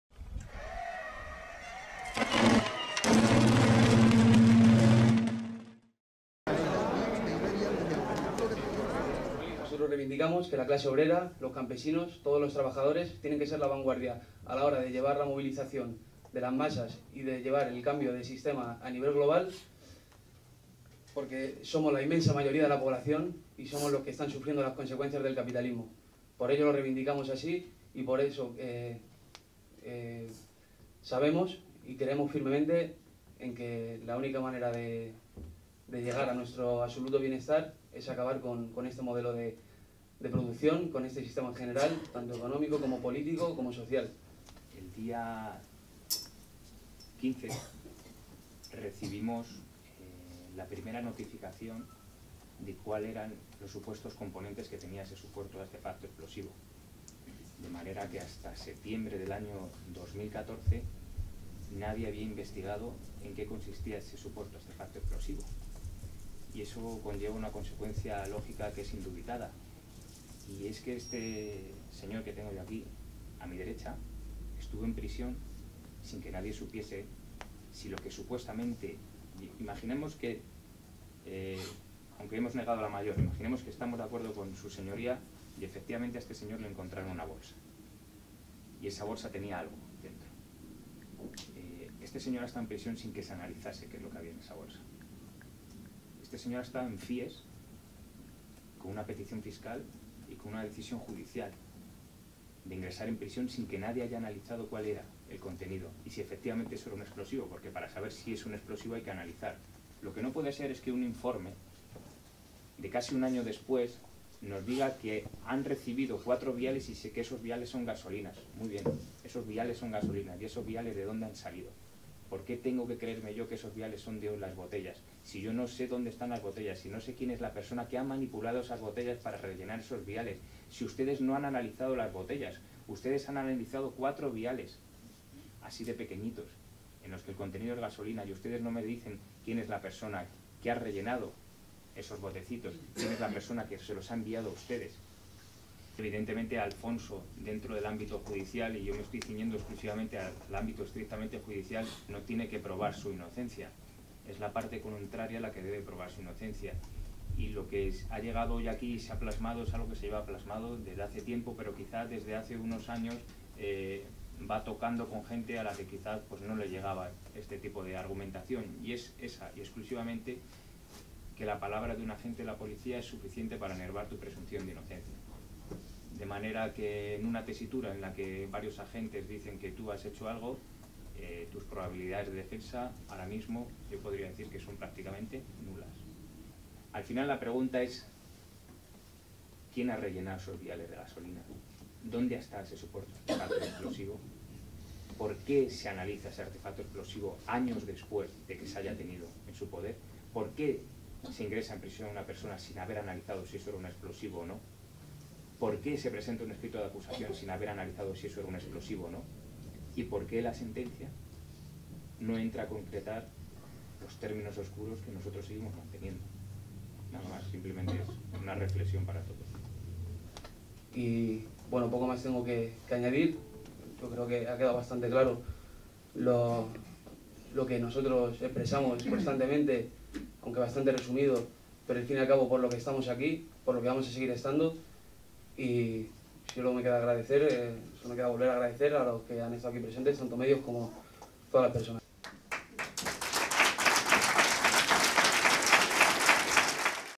Alfon no ingresará en prisión hasta que no se resuelva el recurso ante el Supremo (Rueda de Prensa)
Madrid, 16 de enero de 2015. Erlantz Ibarrondo, abogado de Alfonso Fernández Ortega, recurrirá ante el Tribunal Supremo la sentencia de la Audiencia Provincial de Madrid que condena al joven a una pena de 4 años por tenencia de explosivos. La defensa basará el recurso en las contradicciones en que los agentes de la Policía Nacional incurrieron en el juicio del pasado 25 de noviembre, así como en la ruptura de la cadena de custodia del artefacto explosivo que presuntamente portaba Alfon en la huelga general del 14 de noviembre de 2012.

El abogado ha denunciado esta mañana en el Ateneo de Madrid que se está utilizando la práctica de la “prueba diabólica, conforme a la cual el imputado debe demostrar que es inocente en vez de que la parte contraria tenga que demostrar su culpabilidad”. No se ha podido demostrar, denuncia Ibarrondo, “que la mochila que se analizó fuera portada por Alfon en ningún momento”. El abogado señala además que nadie les explica quién entregó a la policía científica los viales con gasolina -en teoría extraídos de las botellas que conformaban el explosivo-. “La brigada que recibe el artefacto inicialmente es diferente a la que se lo entrega a la policía científica, pero esto no ha sido relevante para la sala. ¿Dónde ha estado el artefacto todo este tiempo?”. Tampoco han sido prácticamente tenidos en cuenta por la sala los testigos presentados por la defensa: "no han sido referenciados en la sentencia", denunica Ibarrondo, que lamenta que "la palabra de un agente de Policía sea suficiente para enervar la presunción de inocencia".

Erlantz Ibarrondo ha recordado además que las 4 huellas identificables que el informe pericial lofoscópico encontró en el artefacto no han sido analizadas. Ni la fiscalía, ni la jueza, ni la defensa lo han solicitado. En el caso de la defensa, según ha declarado Ibarrondo a DISO Press, ha sido "porque el gasto económico que supone se escapa a nuestras posibilidades".

“Salvo que haya sorpresas”, ha dicho el abogado, Alfon no ingresará en prisión hasta que no se resuelva el recurso. “Recibimos la sentencia el día 8 de enero y el 9 [la fiscalía] estaba pidiendo su ingreso en prisión, pero la sala ha decidido que no tiene que ingresar porque no existe riesgo de fuga, como demuestra las declaraciones que ha ido haciendo el día 15 de cada mes durante todo este tiempo”, ha explicado el abogado. En la comparecencia de esta mañana, Ibarrondo ha recordado también que Alfon fue encarcelado de manera preventiva en régimen FIES, a petición del Minsiterio Público, "sin que se hubieran analizado los componentes del supuesto artefacto".

Desde la Plataforma Alfon Libertad denuncian que se ha tratado al joven “como cabeza de turco, como advertencia para todos los que luchan con la terrible situación en la que nos encontramos”. También en opinión del propio Alfon, "las motivaciones son políticas". Su abogado confía ahora en que el recurso tenga "posibilidades de prosperar".